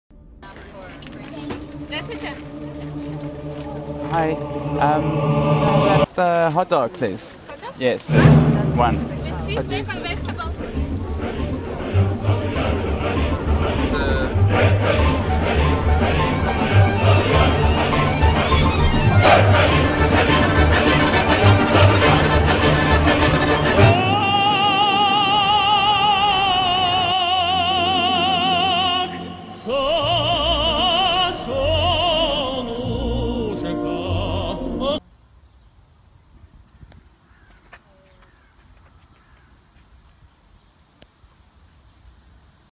Hot Dogs in Moscow
Never realised how popular hot dogs would be in Moscow